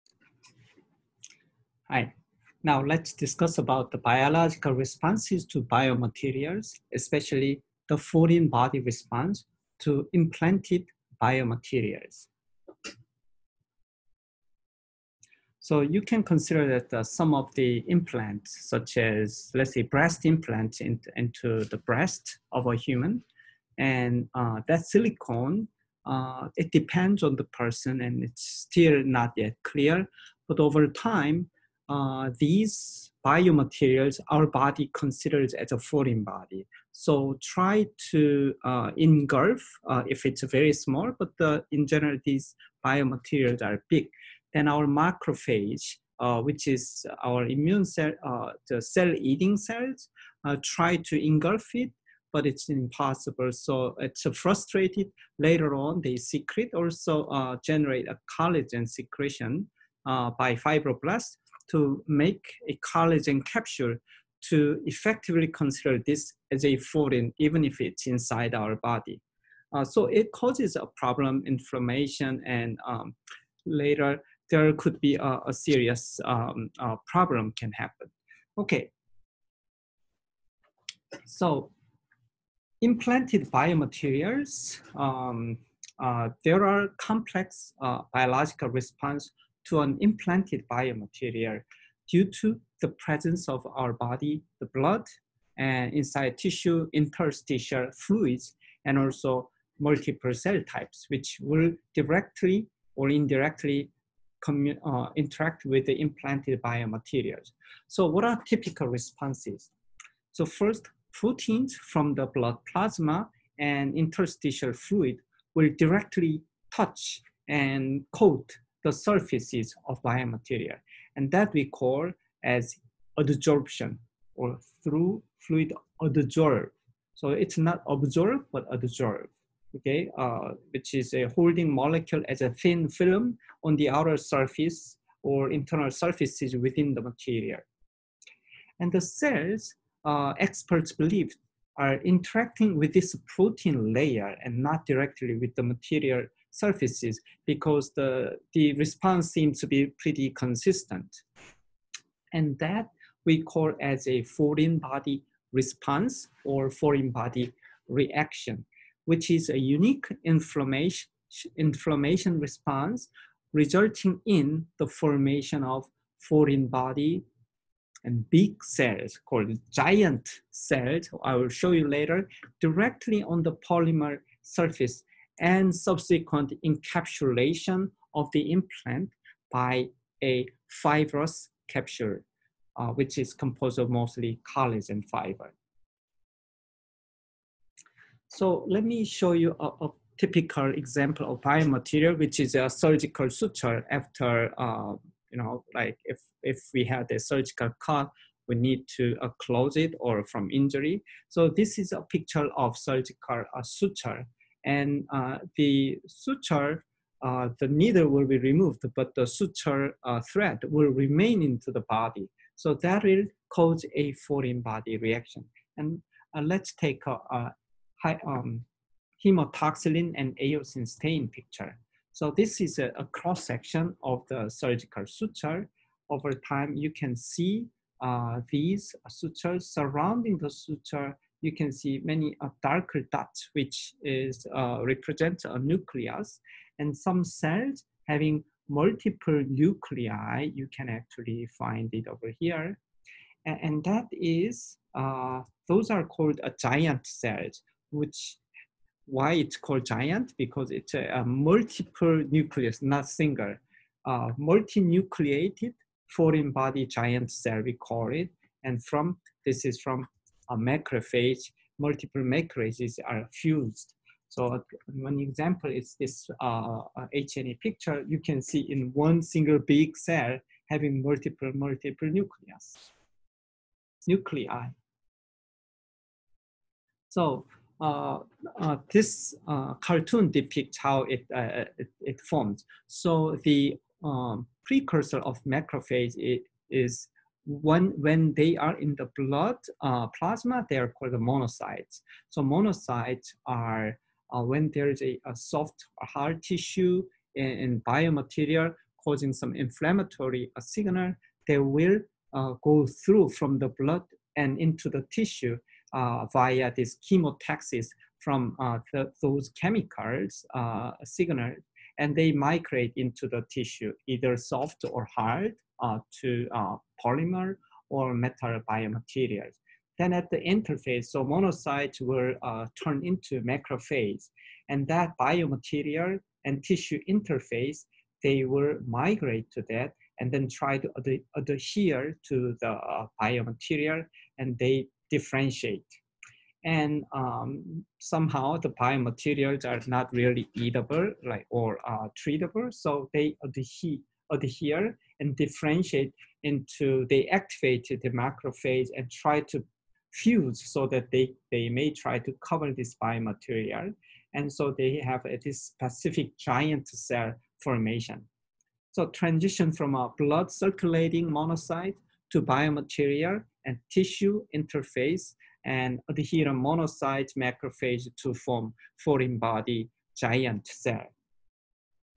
9.5 Biomaterials: FOREIGN BODY RESPONSE (FBR)
Professor Euiheon Chung presents the nuts and bolts of Medical Engineering. The application of fundamental engineering principles to solve outstanding problems in biology and medicine is covered. This course is inspired by Prof. W. Mark Saltzman from Yale University and covers the majority of his textbook (Biomedical Engineering: Bridging Medicine and Technology, Cambridge University Press, ISBN-10: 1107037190)